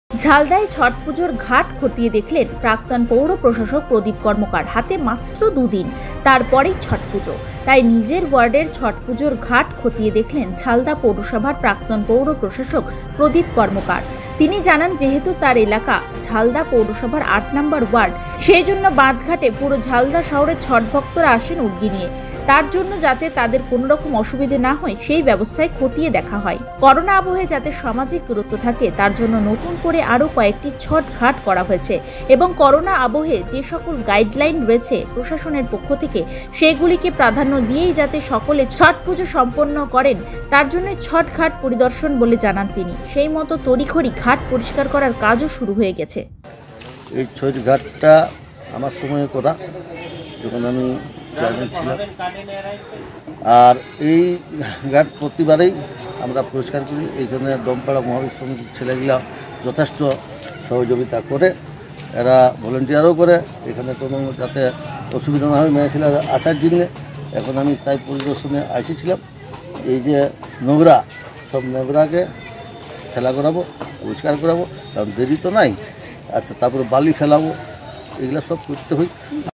ছটপুজোর ঘাট খতিয়ে দেখলেন প্রদীপ কর্মকার
Purulia: - Former Municipal Administrator Pradeep Karmakar inspected Chhatpujo Ghat in Jhalda.
Only two days in hand, then Chhat Pujo. So Pradeep Karmakar, the former municipal administrator of Jhalda municipality, inspected the Chhatpujo ghat in his ward.
He said that his area is Ward No. 8 of Jhalda Municipality. That is why Chhat fans from all over Jhalda come to the dam with soup on their heads to give orgy. So he checked the system so that they would not have any problem.
 A few more new ghats have been constructed in Corona to keep the social distance.
He also said that the administration has given priority to all the guidelines in Corona Abe so that the Chhat Puja is performed for all. The work of cleaning the ghat also started soon.